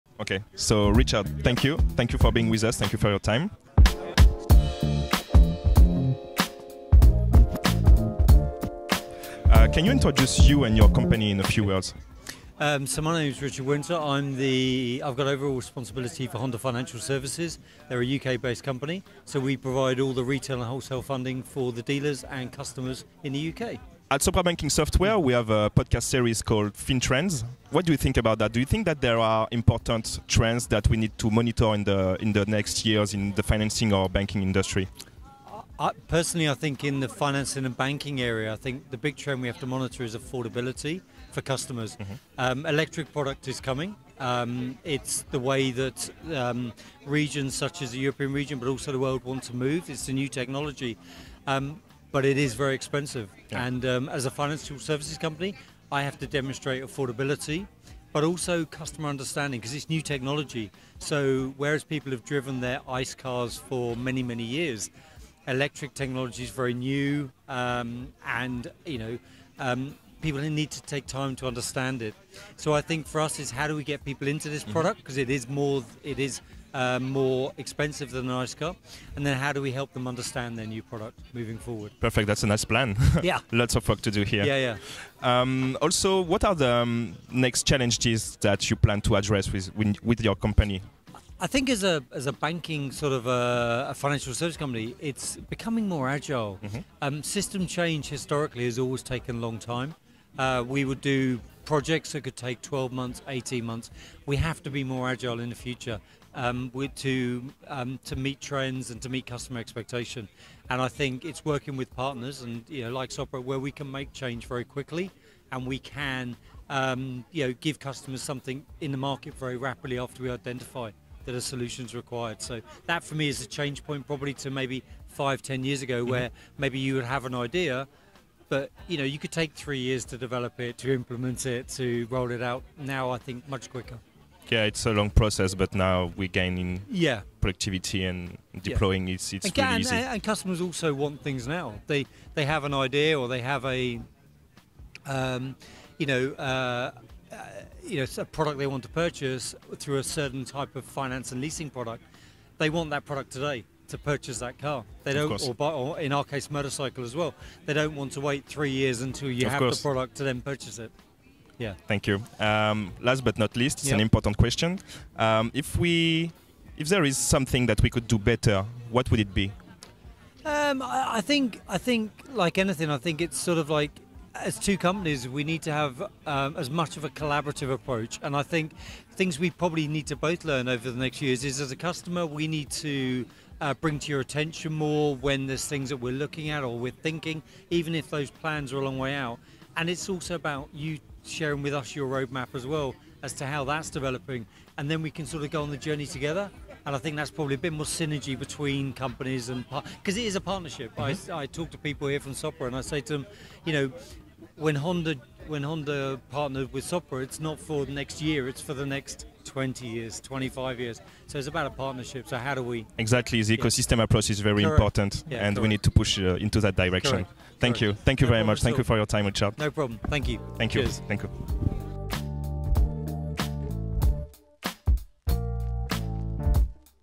FinTalks | Richard Winter, Operations Director at Honda Finance Europe Plc
Meet Richard Winter, Operations Director at Honda Finance Europe Plc trading as Honda Financial Services and part of the Honda Motor Company. The company's aim is to provide UK customers with the most appropriate and convenient finance solution to enable them to buy a new Honda car.